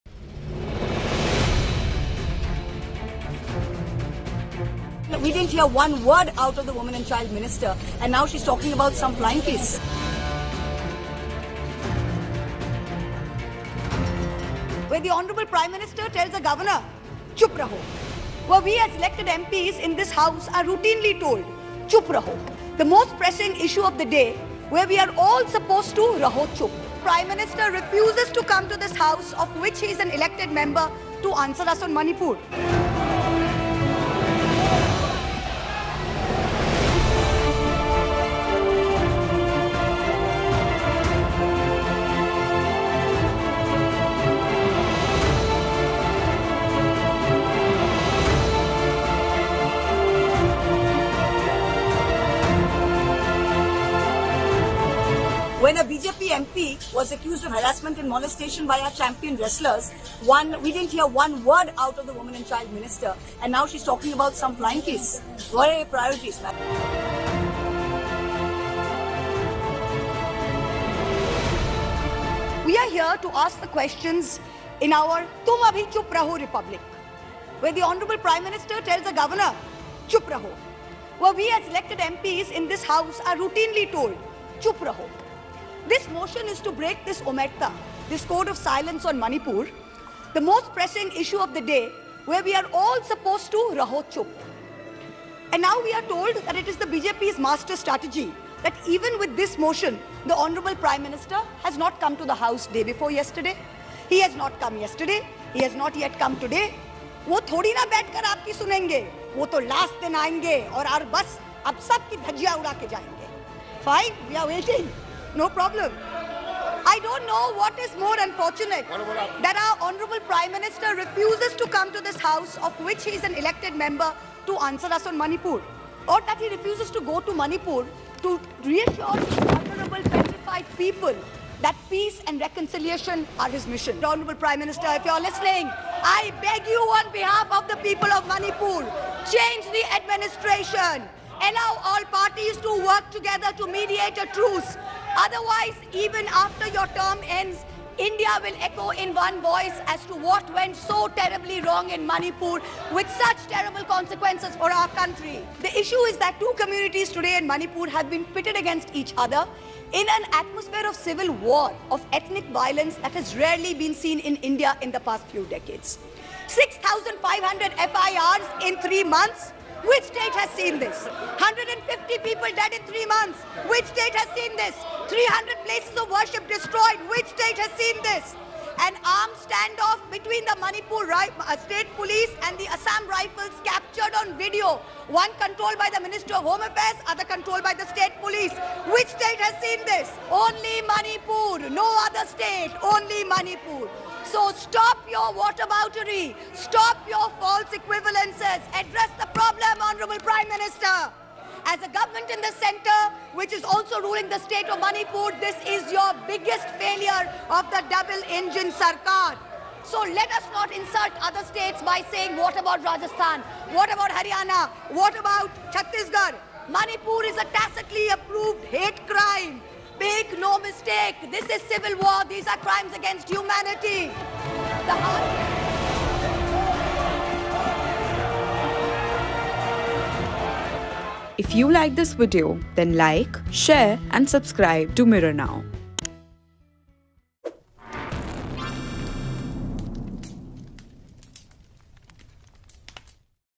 Mahua Moitra in Lok Sabha | TMC MP Attacks PM Modi, Smriti Irani Over Manipur | Rahul Gandhi Speech
Mahua Moitra in Lok Sabha | Huge ruckus in Lok Sabha after Rahul Gandhi's 'flying kiss' row. TMC MP Mahua Moitra backed the Congress leader in the controversy. She attacked the BJP govt and PM Modi on violence against women in Manipur. She questioned the presence of the WFI Chief in the parliament. The MP also questioned PM Modi over the 'chup raho' statement. Moitra said the no-confidence motion is to break the code of silence on Manipur. Watch the video here